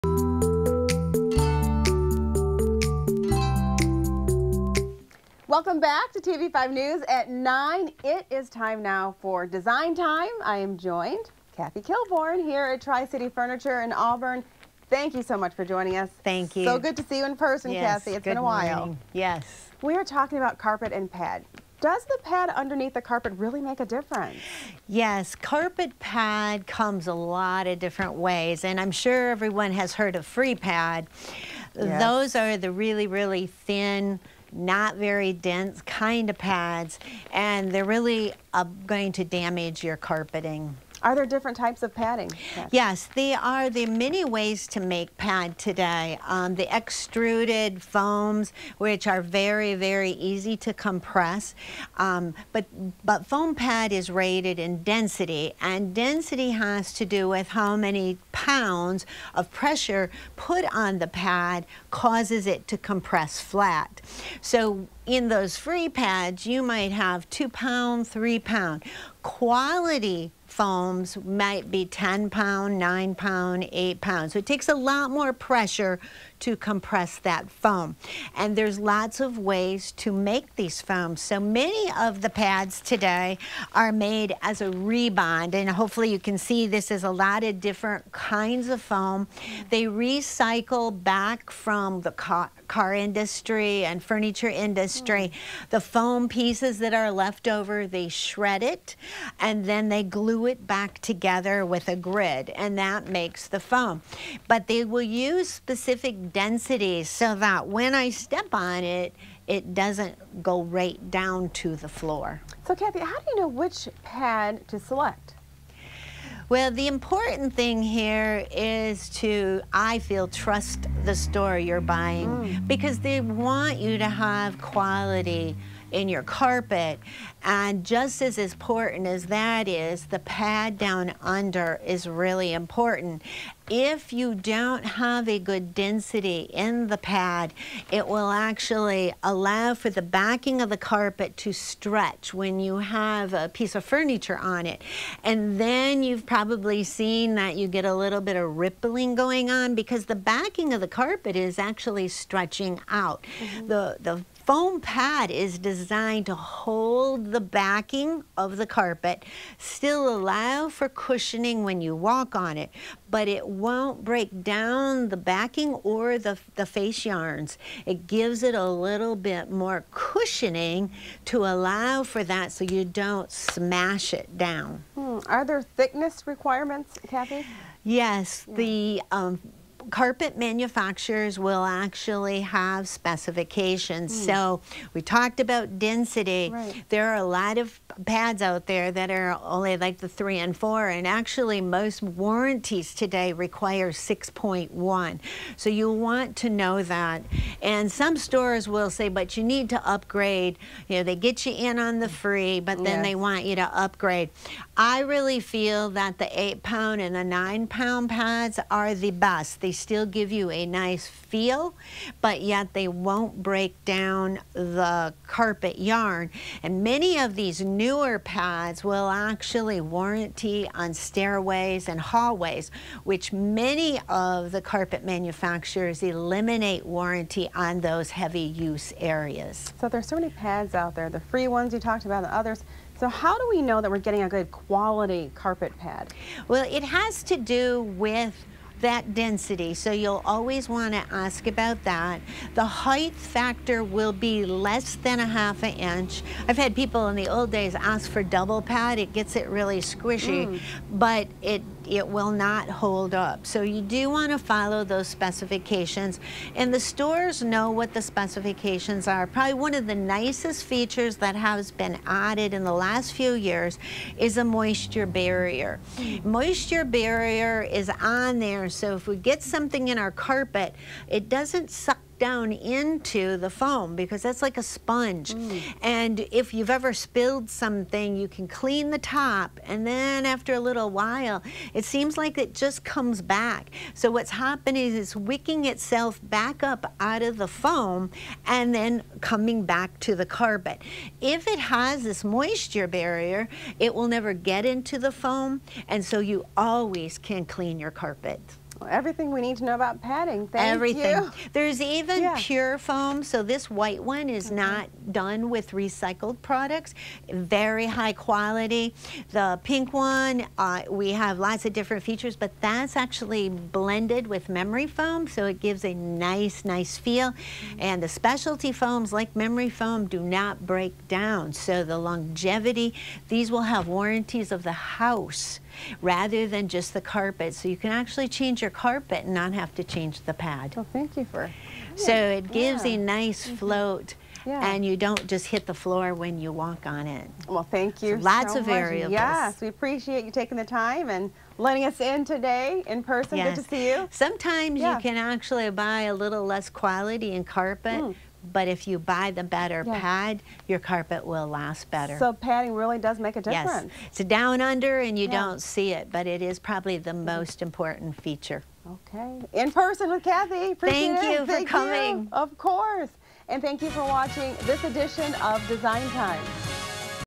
Design Time: Carpet padding
In this Design Time we take a look at the importance of carpet padding.